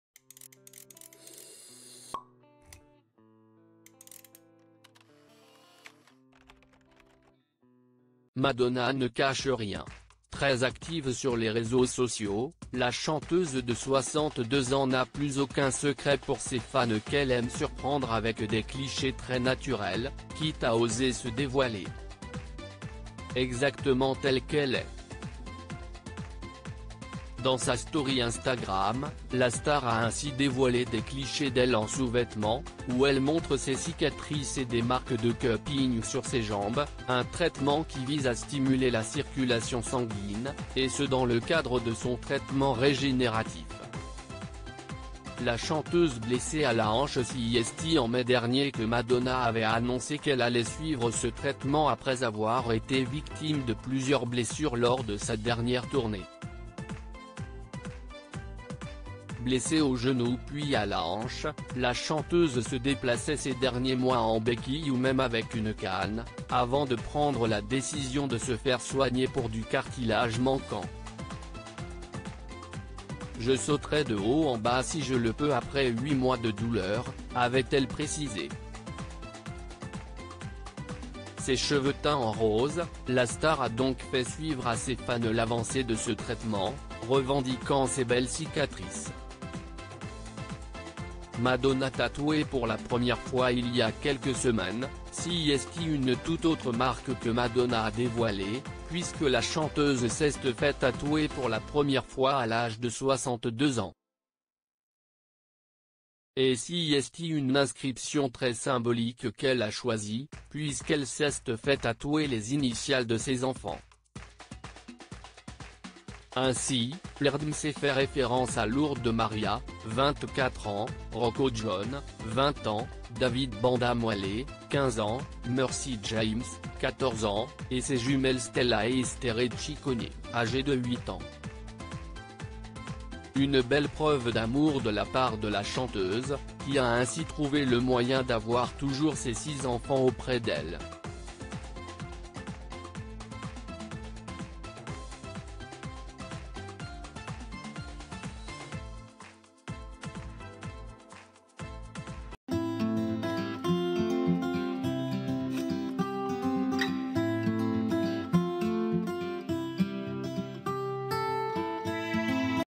Madonna : à 62 ans, la chanteuse dévoile une impressionnante cicatrice lors d'une séance photos en s
Madonna : à 62 ans, la chanteuse dévoile une impressionnante cicatrice lors d'une séance photos en sous-vêtements